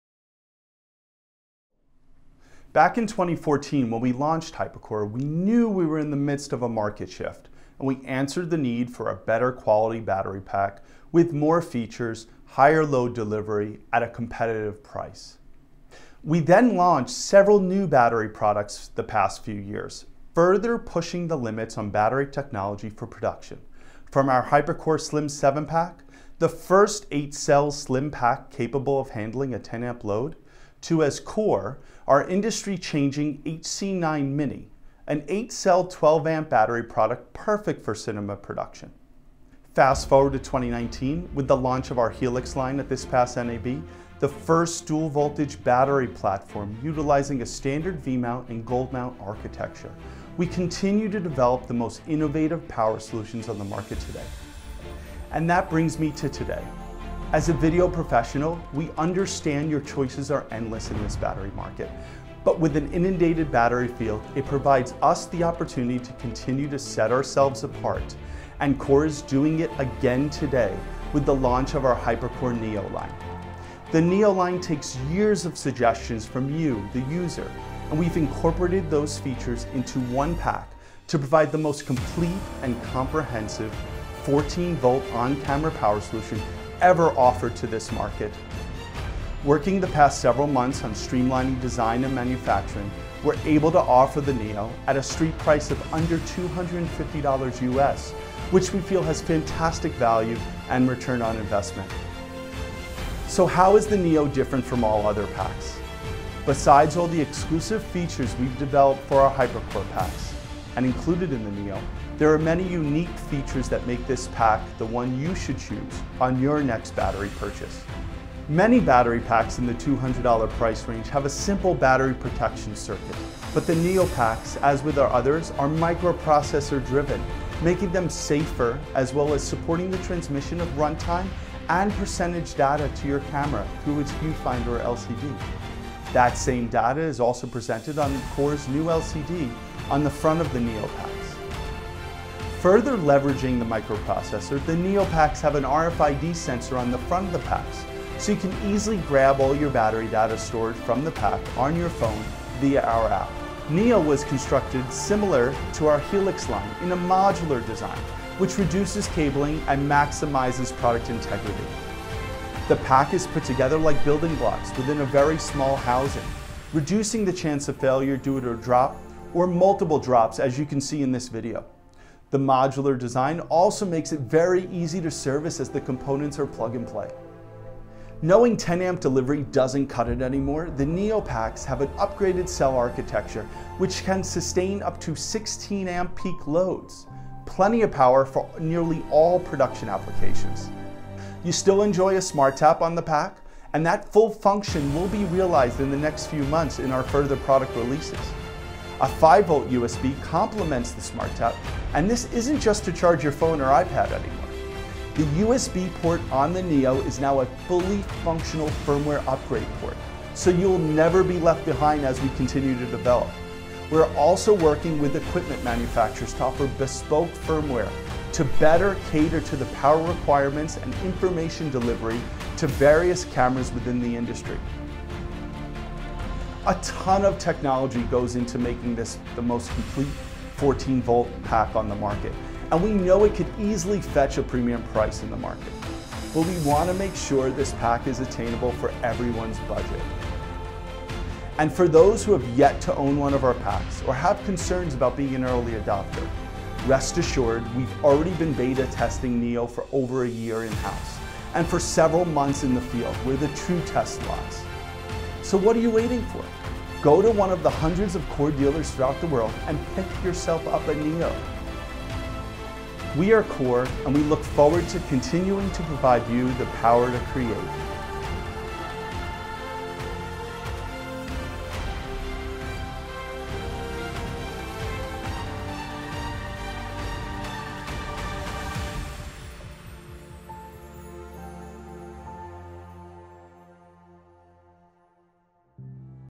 NEO Full Circle
Since 2014, Core SWX has been redefining battery technology with the launch of Hypercore, to Hypercore Slim and Mini, then the Helix series, and now Hypercore NEO.

Hypercore NEO is the next generation of power solution for the cine and broadcast market and this video explains why.